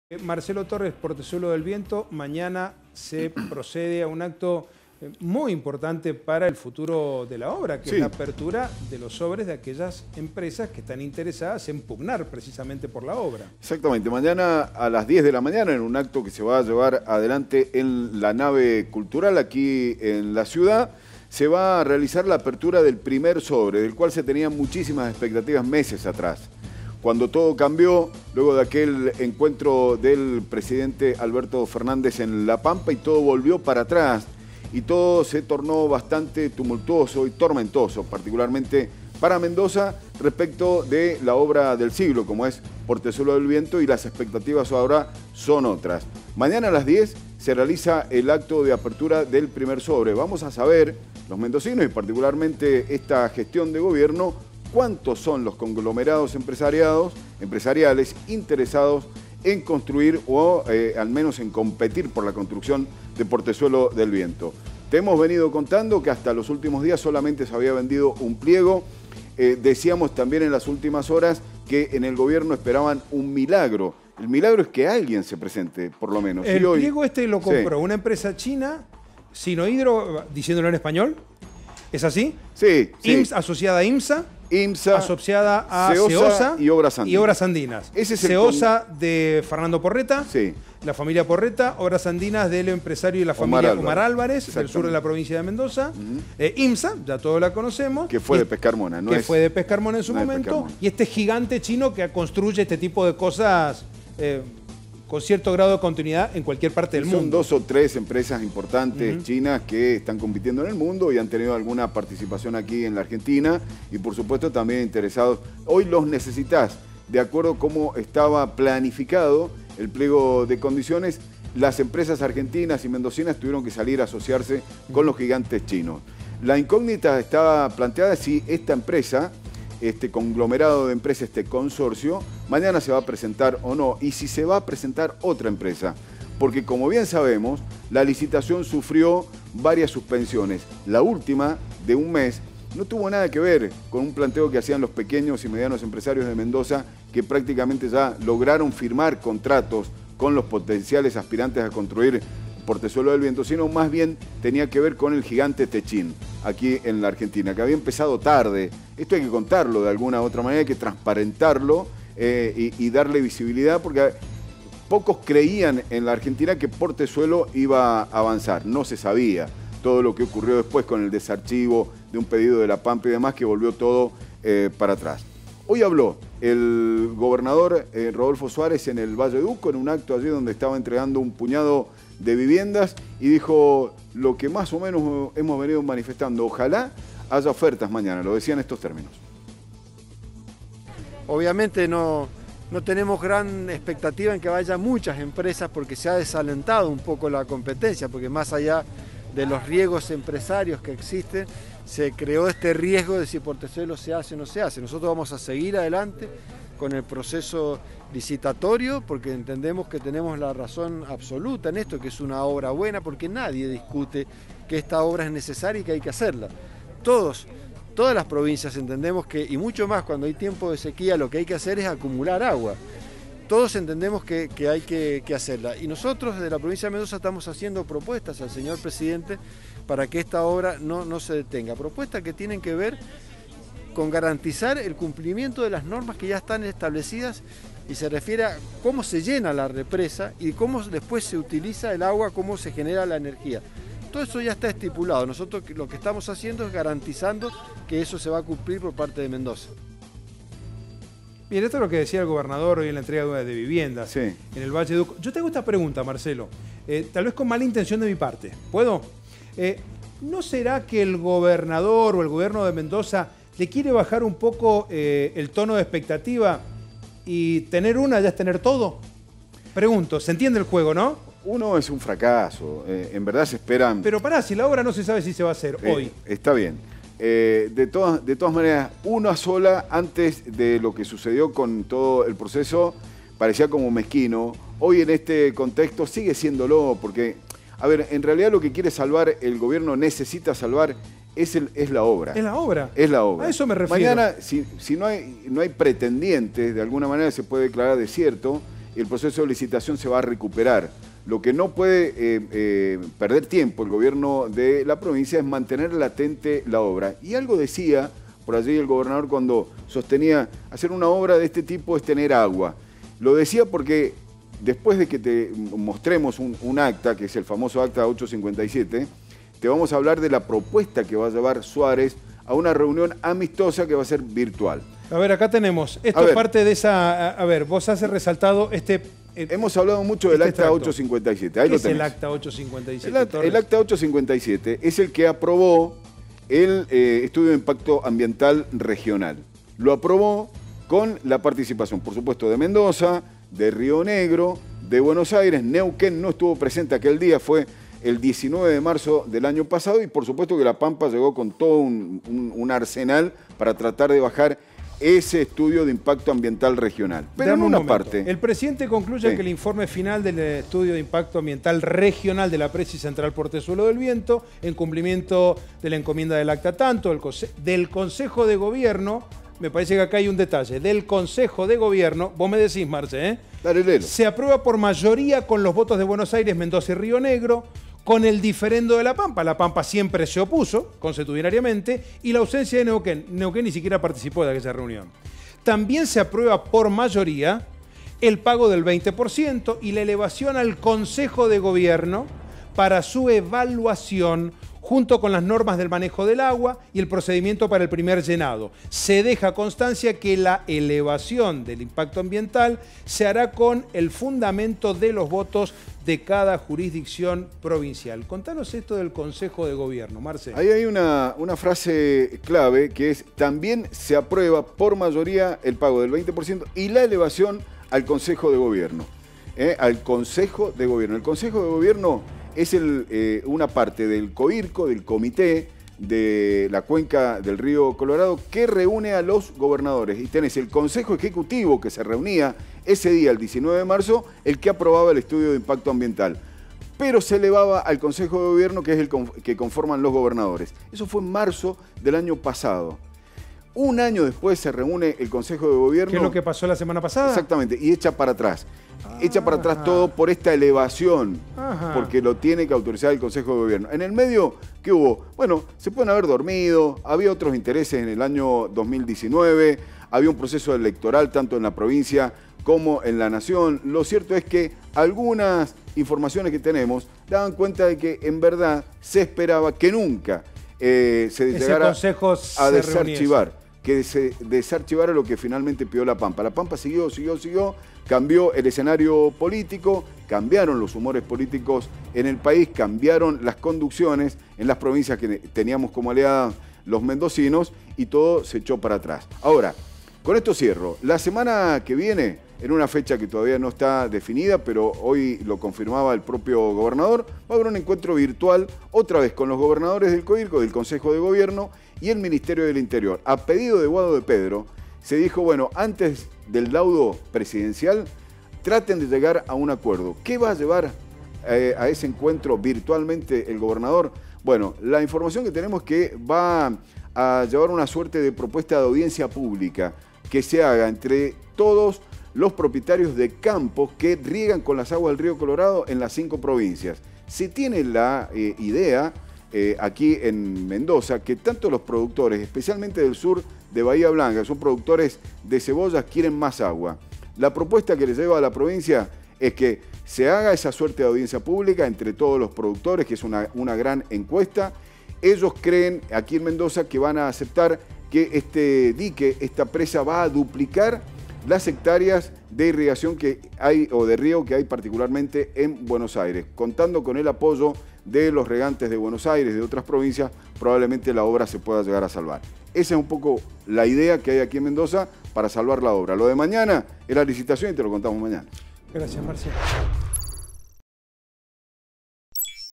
Portezuelo del Viento: Suárez espera que la licitación se desarrolle y que la obra siga su curso
A pocas horas de la apertura de los sobres, la Provincia espera que el proceso siga su curso a pesar del pedido del COIRCO de realizar un nuevo informe de impacto ambiental.